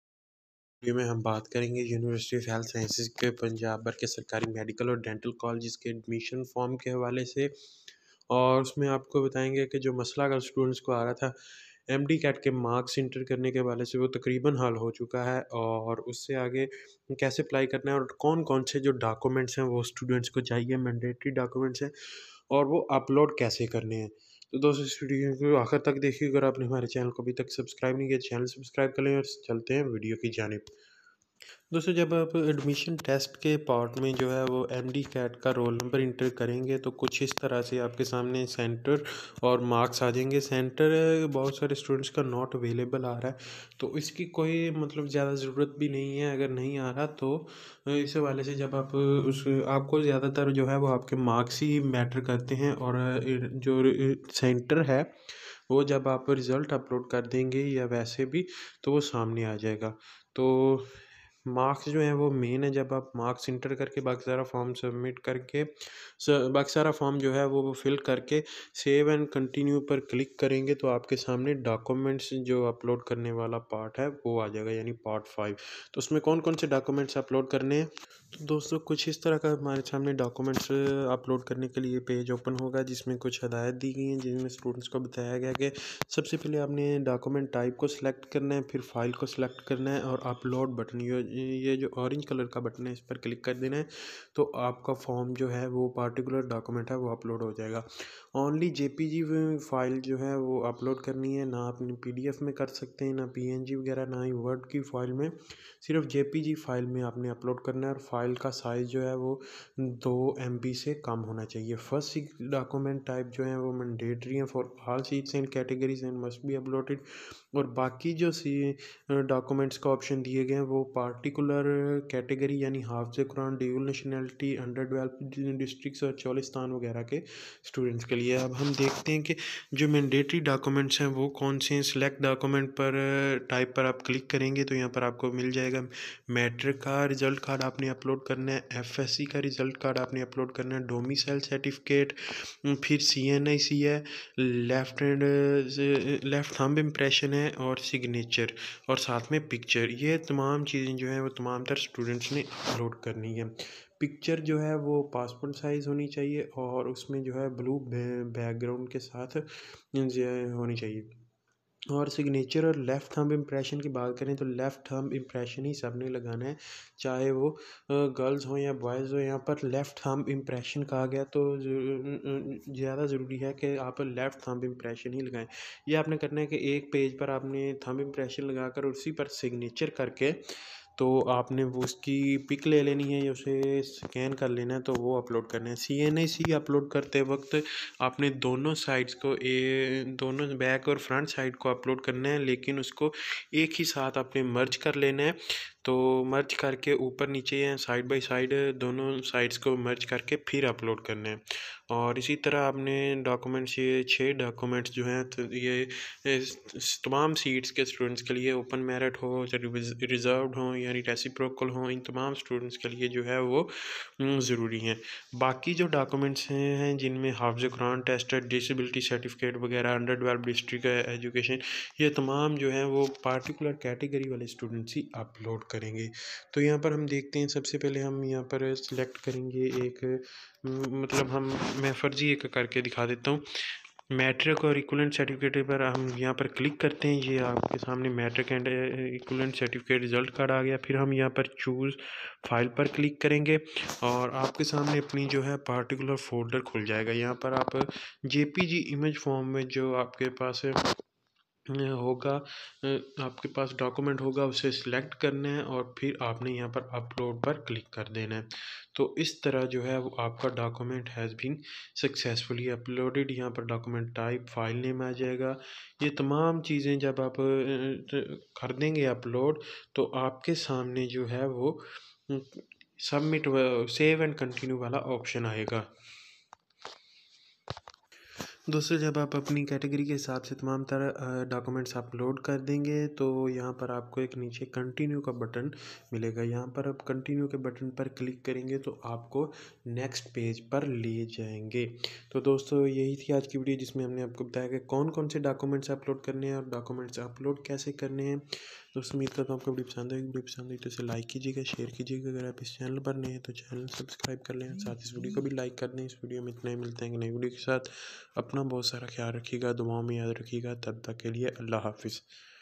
DOCUMENTS REQUIRED FOR UHS ADMISSIONS 2020 - HOW TO UPLOAD - FORMAT - MDCAT MARKS ISSUE RESOLVED
How to upload documents for uhs govt mbbs/bds admissions 2020